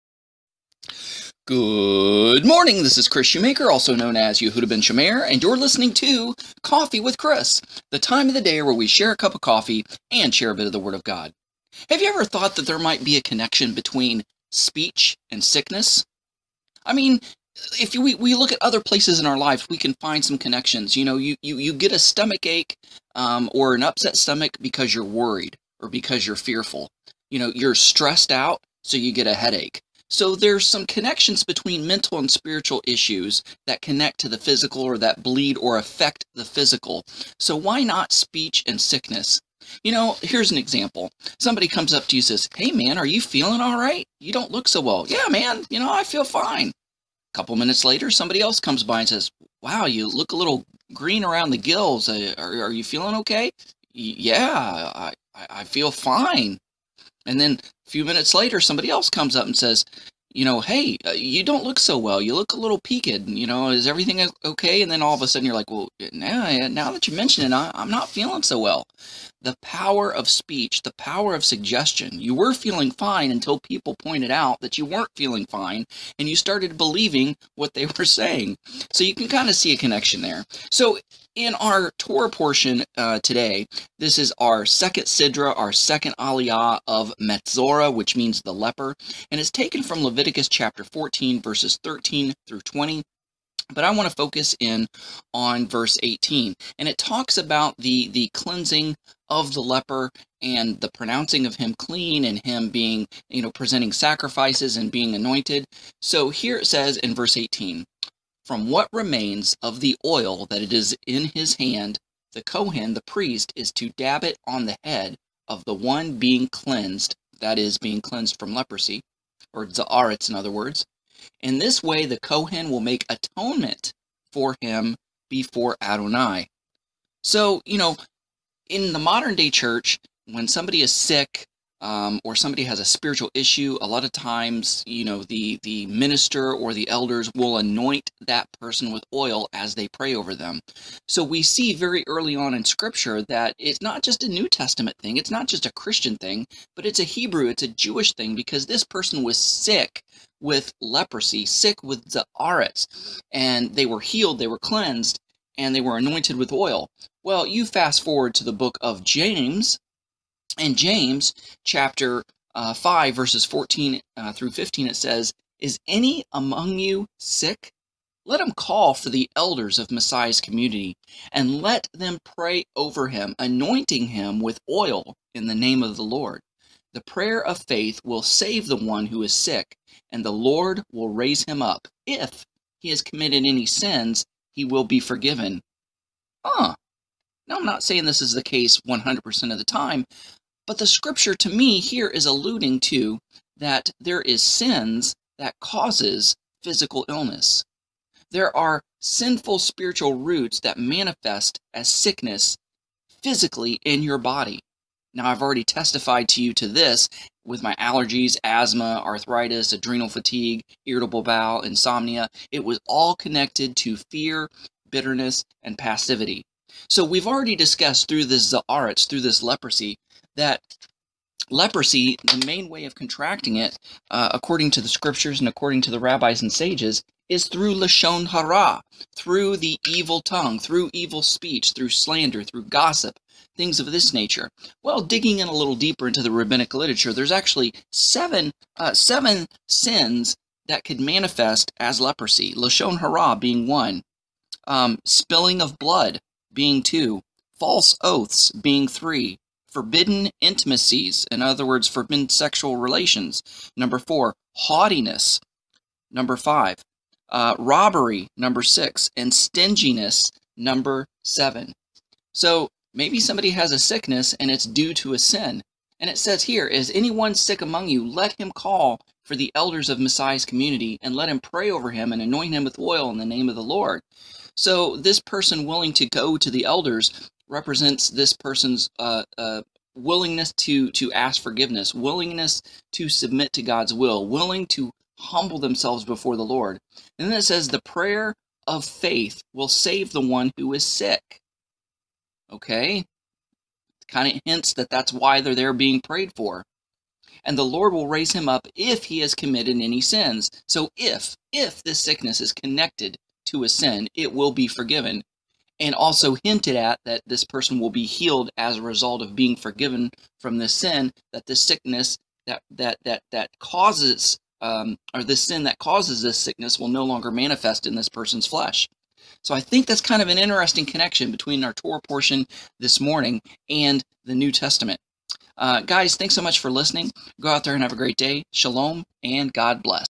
CWK: Monday Metzora “Speech and Sickness”
Coffee with Kris: Monday Metzora “Speech and Sickness”
Lev. 14:13-20
Kris Shoemaker – Yehudah ben Shomeyr